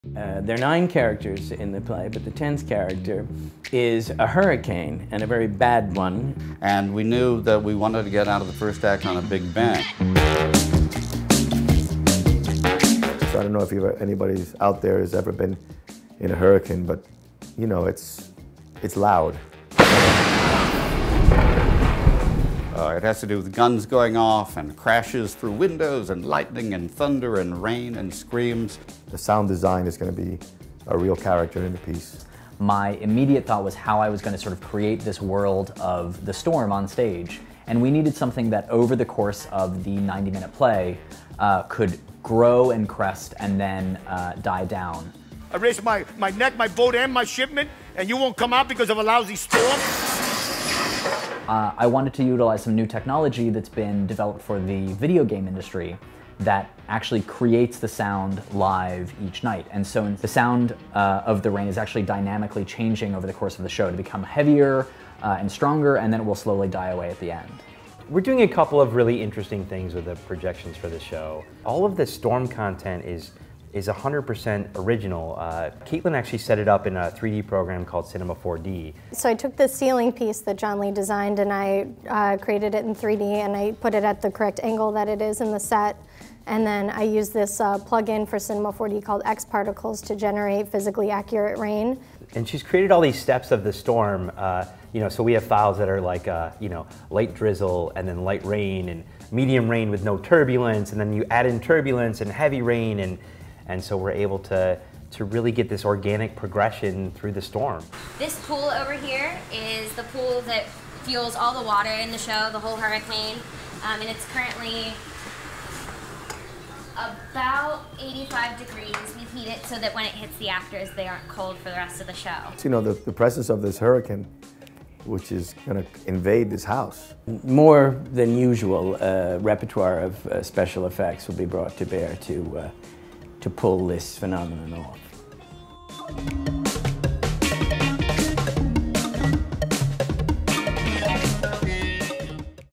Making a Hurricane: Key Largo at the Geffen Playhouse
Meet the creative team behind the category 5 hurricane in the Geffen Playhouse production of "Key Largo" and discover how they make the storm onstage every night. From lighting to sound design, video projections to backstage water tanks; discover the secrets to making a realistic theatrical hurricane in "Key Largo."

Welcome to the eye of the storm! Key Largo is a bold reimagining of Maxwell Anderson’s Broadway hit that became the iconic noir film starring Humphrey Bogart and Lauren Bacall. Returning from World War II, disillusioned Frank McCloud travels to a hotel in Key Largo to pay his respects to the widow of a fallen friend. What McCloud doesn’t count on is an entirely different battle with mobsters who have overtaken the hotel, led by the ruthless Johnny Rocco (Academy Award nominee Andy Garcia). As a hurricane barrels toward the Keys, McCloud must face his demons in order to take down a monster.

Adapted by Jeffrey Hatcher & Andy Garcia
Directed by Doug Hughes
Produced in Association with Frank Mancuso & Andy Garcia
Original Music by Arturo Sandoval
Featuring Stephen Borrello, Joely Fisher, Andy Garcia, Rose McIver, Louis Mustillo, Danny Pino, Tony Plana, Richard Riehle and Bradley Snedeker
Based on the play by Maxwell Anderson and the Warner Bros. Film
Produced by special arrangement with Warner Bros. Theatre Ventures

World Premiere Adaptation & Geffen Playhouse Commission
KEY LARGO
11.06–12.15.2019